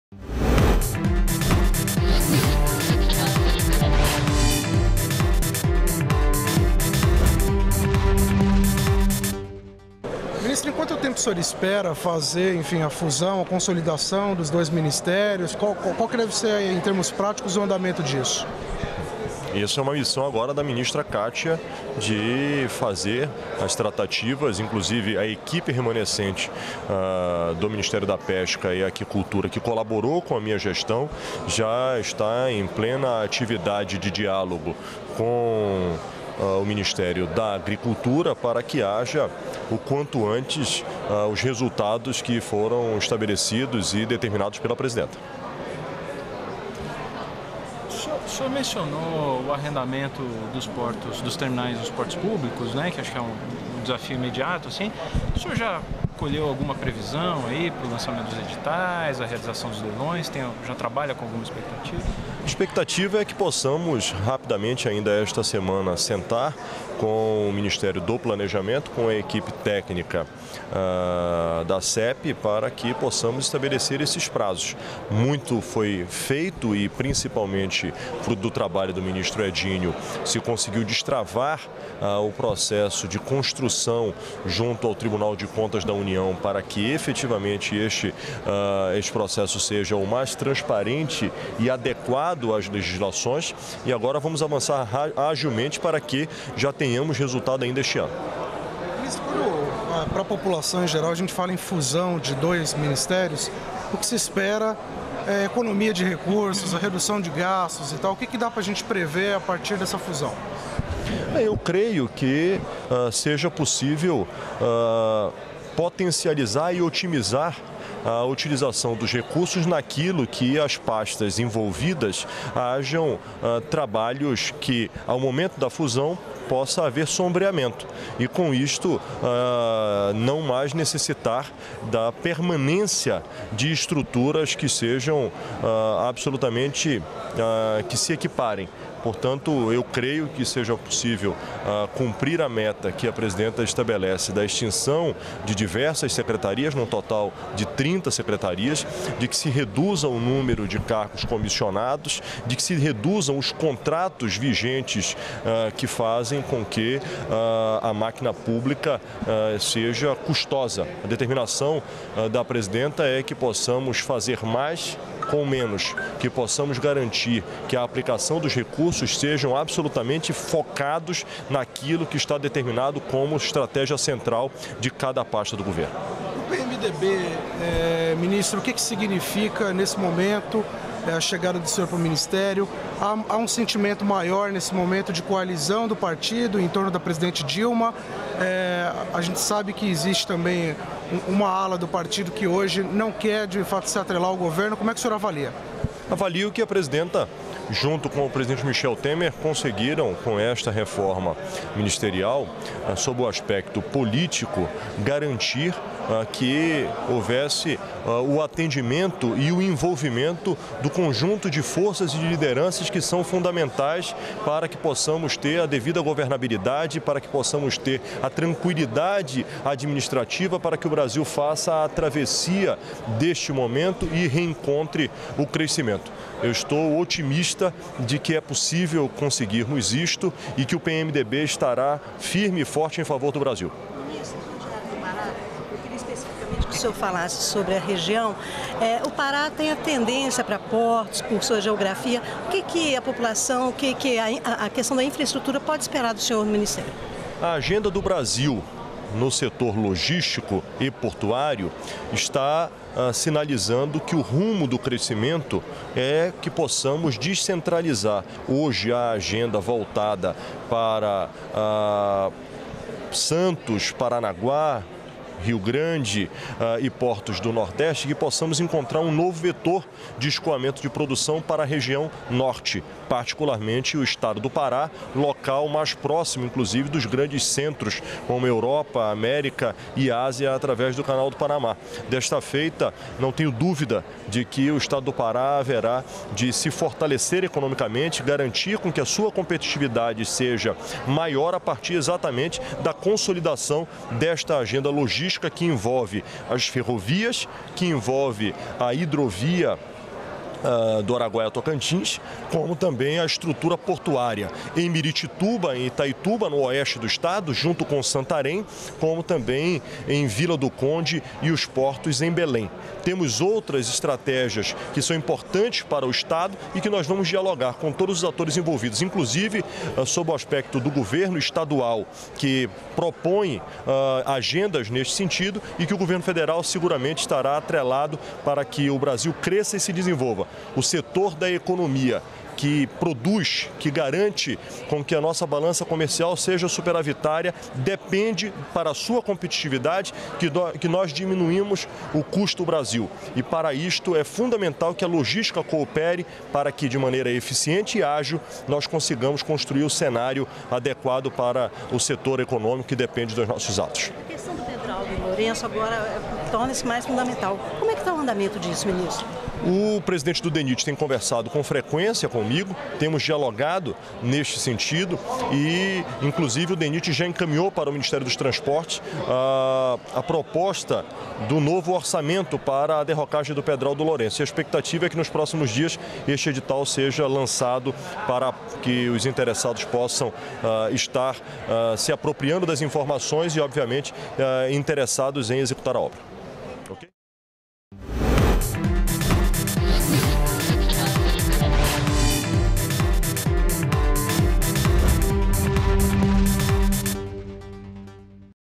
Ministro dos Portos, Helder Barbalho, fala com jornalistas após cerimônia de transmissão de cargos
NOTÍCIAS DA FONTE - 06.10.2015: Ministro dos Portos, Helder Barbalho, fala à imprensa após assumir a pasta e comenta sobre transmissão de atividades de pesca e aquicultura ao ministério da Agricultura.